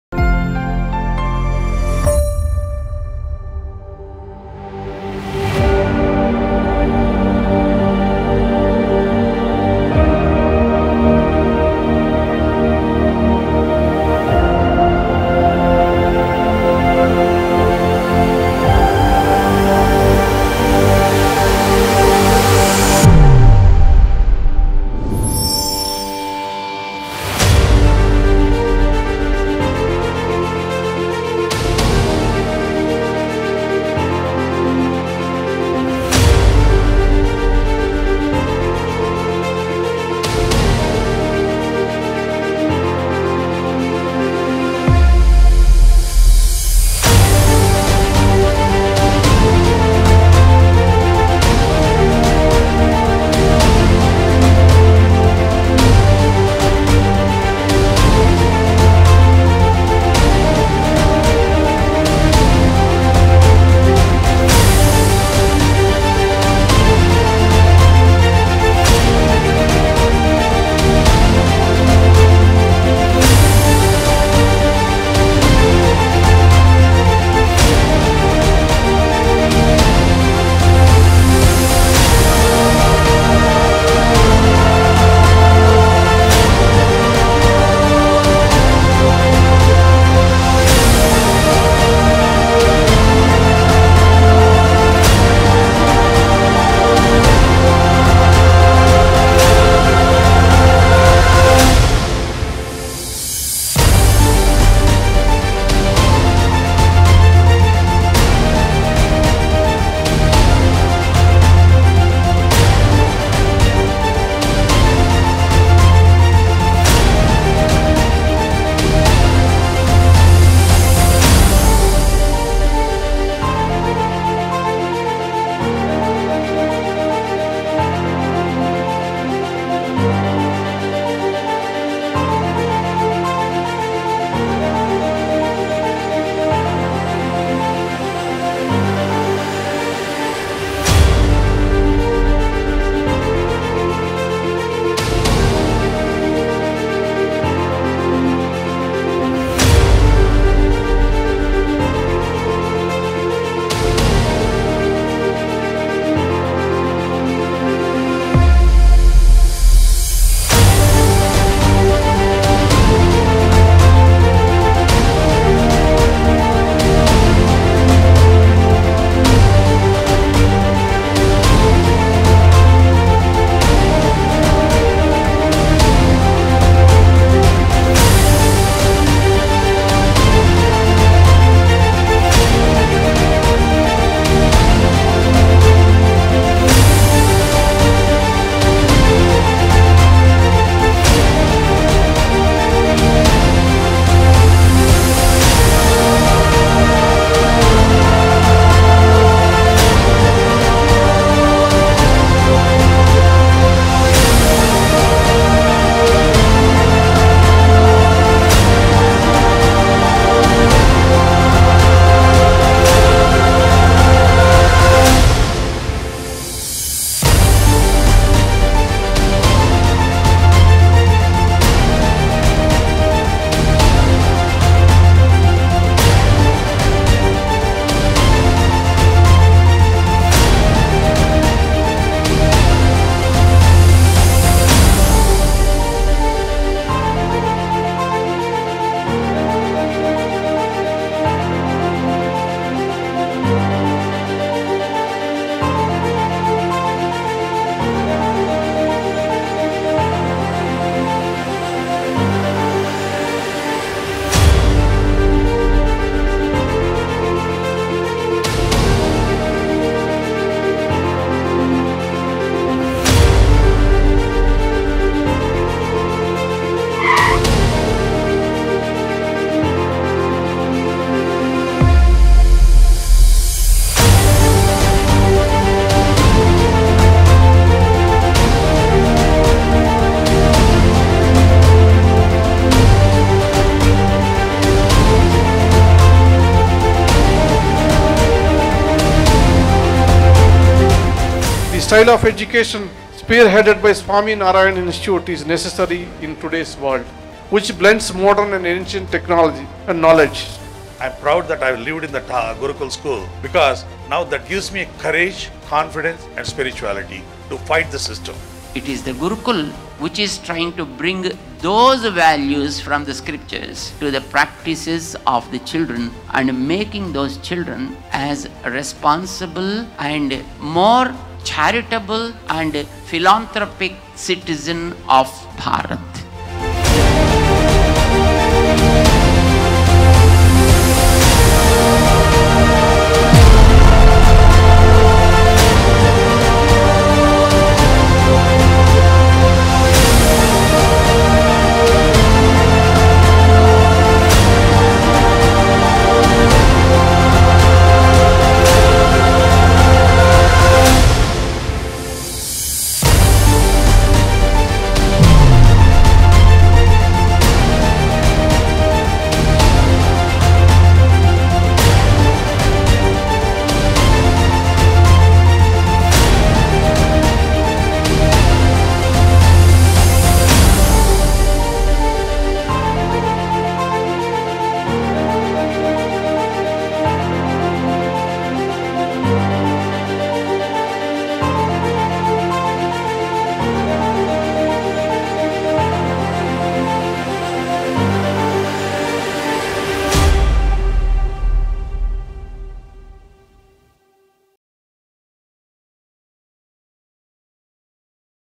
Silver Jubilee Celebration | Highlights | Gurukul Hyderabad | Shree Swaminarayan Gurukul
Highlights of Silver Jubilee Celebration organized on occasion of completing glorious 25 Years of Shree Swaminarayan Gurukul Hyderabad

With Divine presence of
Pujya Gurumaharaj Shri Devkrushnadasji Swami
Pujya Mahant Swami Shri Devprasaddasji Swami
and 50+ Pious Saints

Chief Guests:
Sri Sri Sri Tridandi Chinna Shrimannarayana Ramanuja Jeeyar Swamiji -  Founder of Statue of Equality, JIVA Trust

Justice N. V. Ramanagaru - Honourable Former Chief Justice of India

Dr. Krishna Ella - Founder and Chairman of Bharat Biotech

and many more honourable guests and devotees.